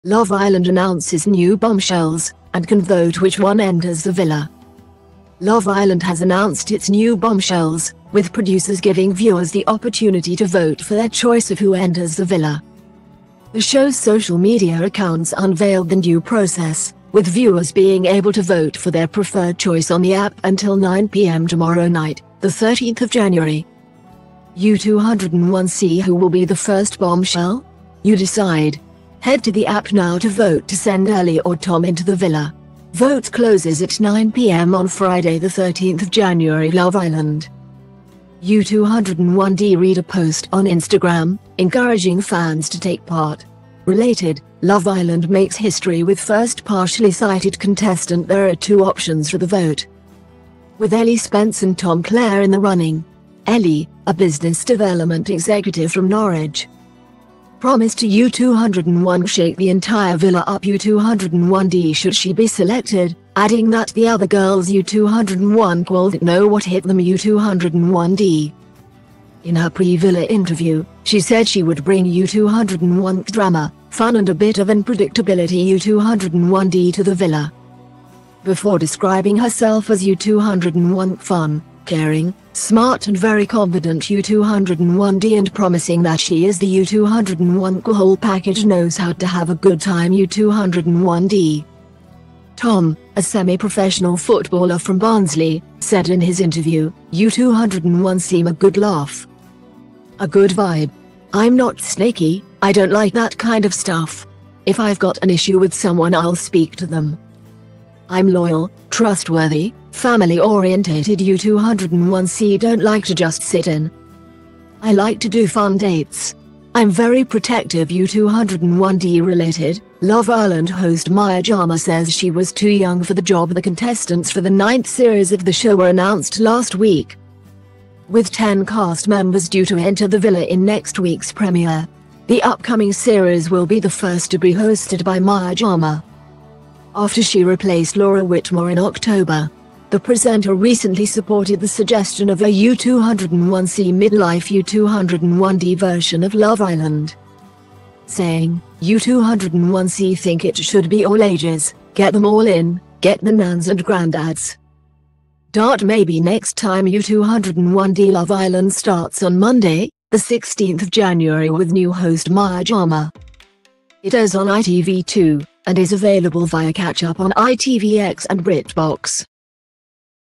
Love Island announces new bombshells – and can vote which one enters the villa
Love Island has announced its new bombshells, with producers giving viewers the opportunity to vote for their choice of who enters the villa .The show's social media accounts unveiled the new process, with viewers being able to vote for their preferred choice on the app until 9pm tomorrow night (January 13).\u201cWho will be the first bombshell? YOU decide. Head to the app now to vote to send Ellie or Tom into the villa! Vote closes at 9pm on Friday 13th January